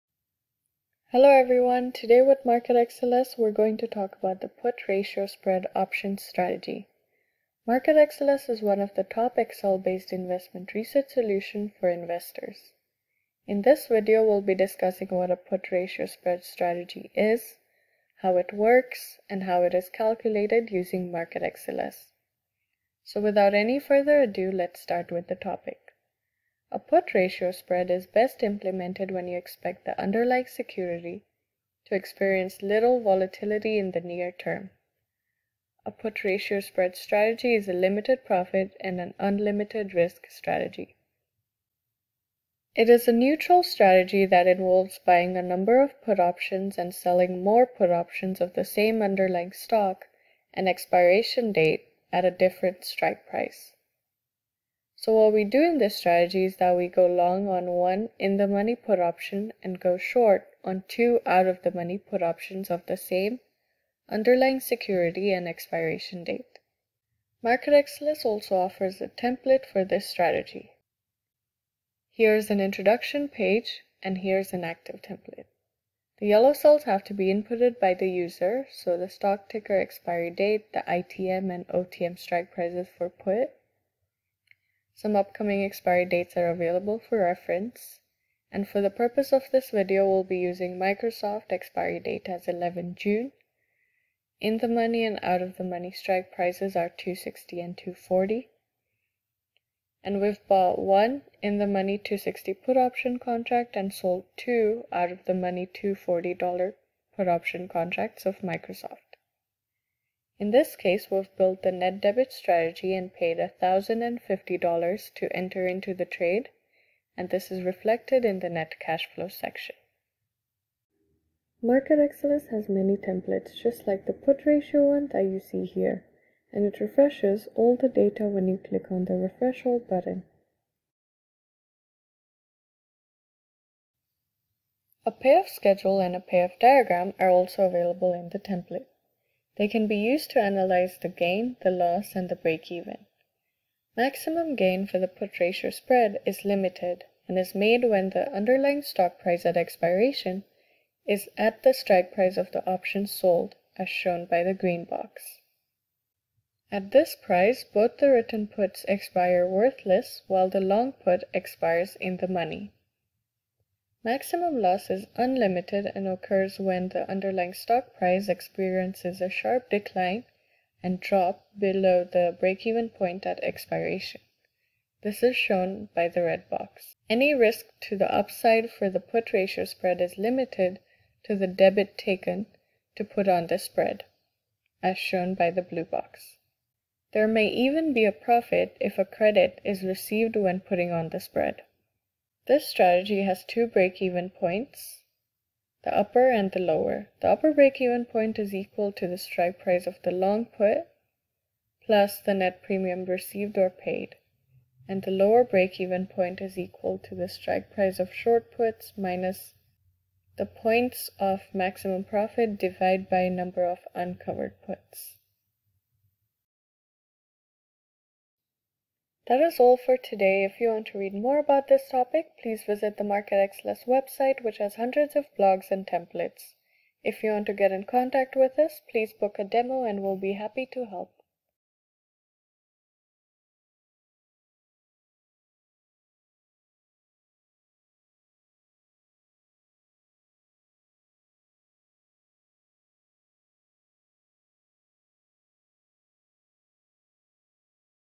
Put Ratio Spread - Options Strategy (Using MarketXLS)
The Put Ratio Spread is a neutral strategy in options trading adopted by traders. The strategy involves buying a select number of put options of a particular stock and expiration date at a higher strike price and selling more options at a lower strike price. It is an unlimited risk options trading strategy which offers a limited profit and is generally opted for by options trader when they believe that the particular stock will not grow and the price will fall, but not more than the strike price in the near term.